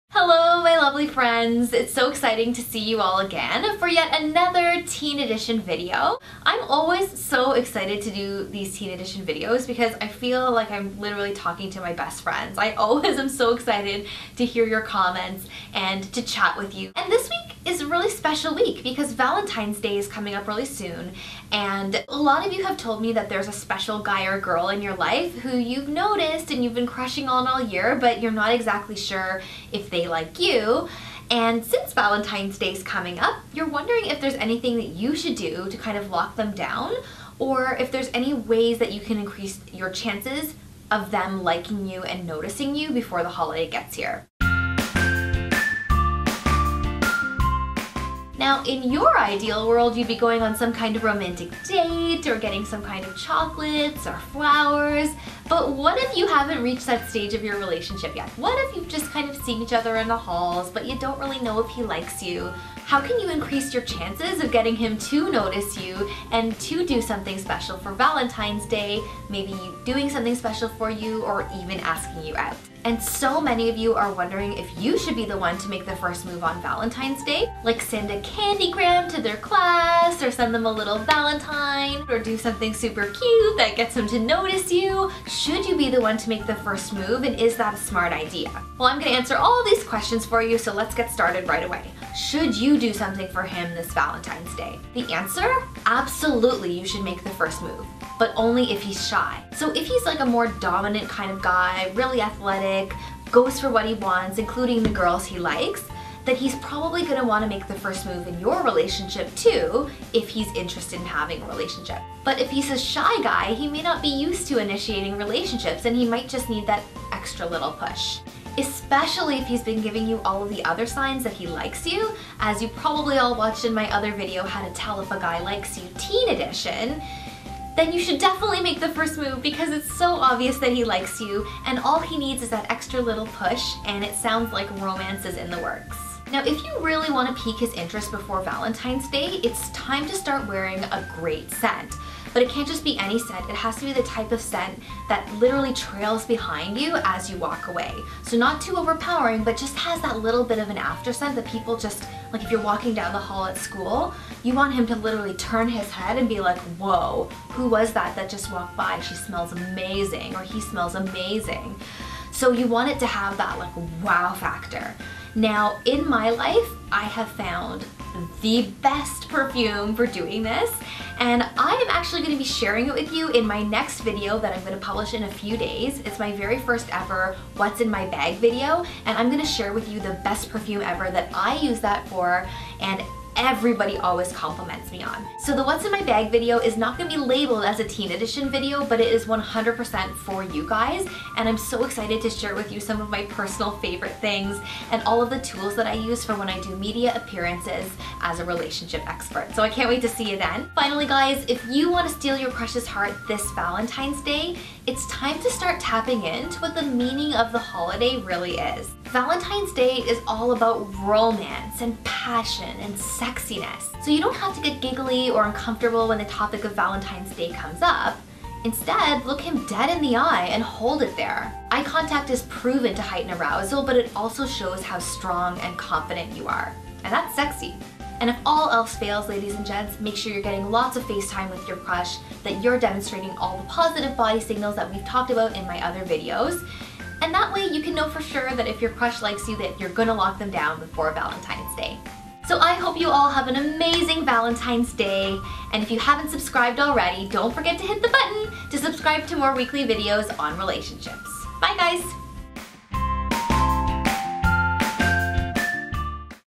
How to Get Your Valentine's Crush:TEEN EDITION
Latest Upload and New Giveaway! "I Copied Cardi B's Maternity Photos... "
How to get your Valentine's Crush gives you my top 3 tips to get the guy or girl you're interested in to notice you, do something special for you, or ask you out for Valentine's Day!